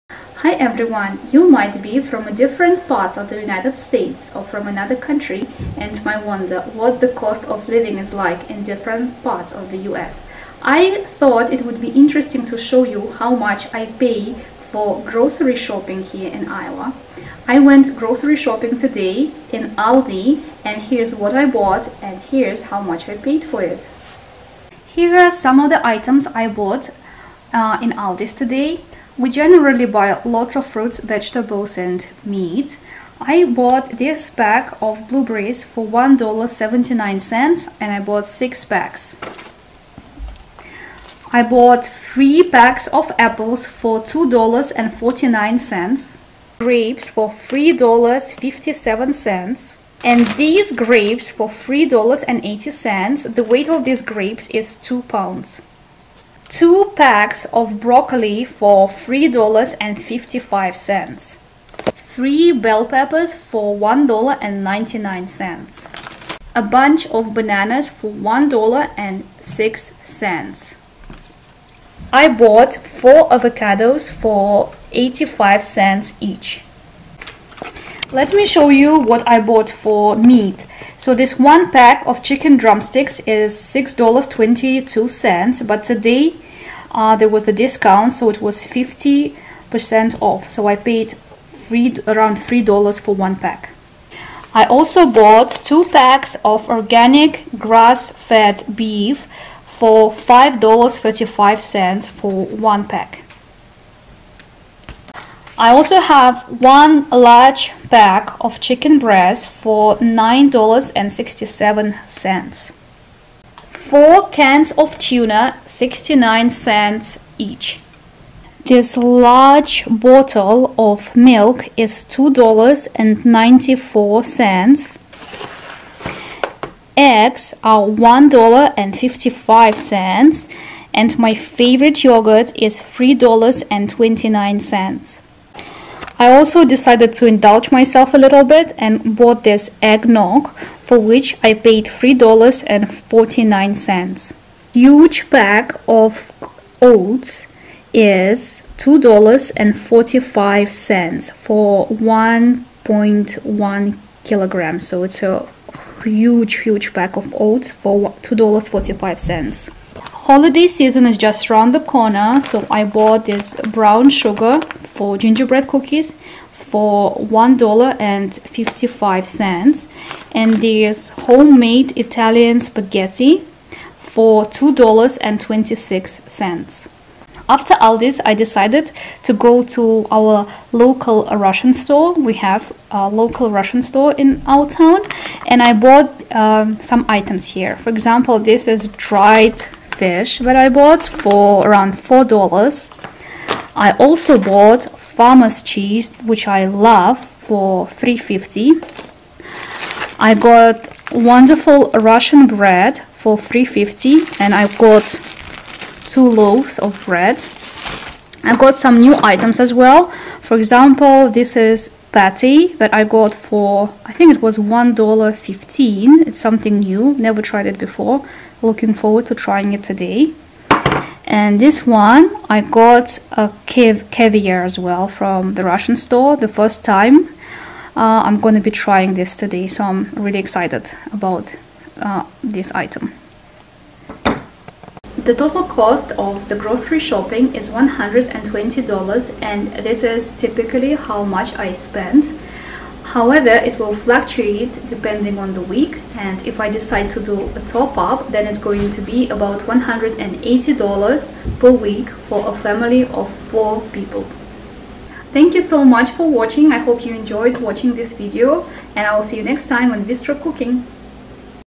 ALDI, IOWA - Cost of Food in Iowa USA 2021
Grocery Shopping in ALDI
Grocery Shopping in Iowa, USA! How much do I pay for my groceries?